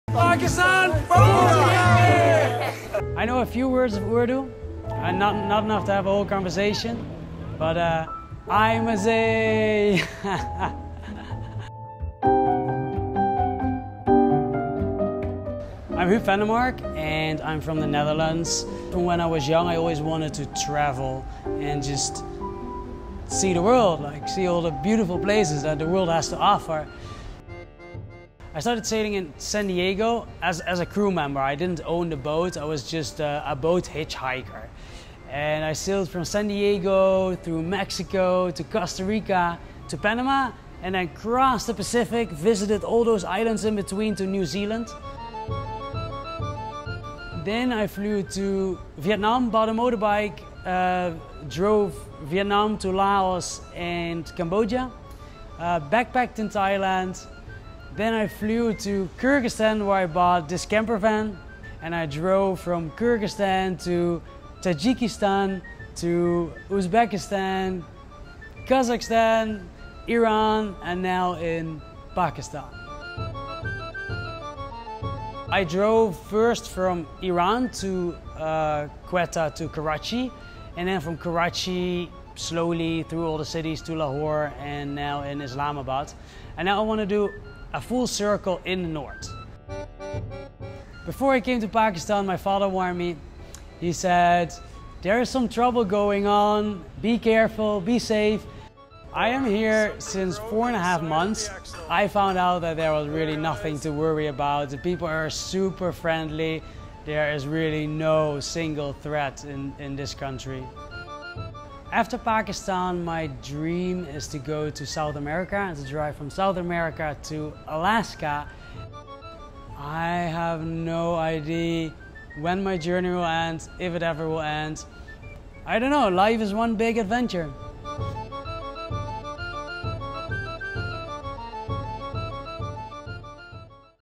ہوب ویندر مارک کا پاکستان اور پاکستانی لوگوں کے بارے میں کیا کہنا ہے؟ - BBCURDU
نیدرلینڈز سے تعلق رکھنے والے ہوب ویندر مارک دنیا کے سفر پر نکلے ہیں۔ وہ مختلف ممالک سے ہوتے ہوئے اپنی کیمپروین پر براستہ ایران پاکستان پہنچے ہیں۔

ہوب کا پاکستان اور پاکستانی لوگوں کے بارے میں کیا کہنا ہے، جاننے کے لیے دیکھیے محمد ابراہیم کی یہ ڈیجیٹل ویڈیو۔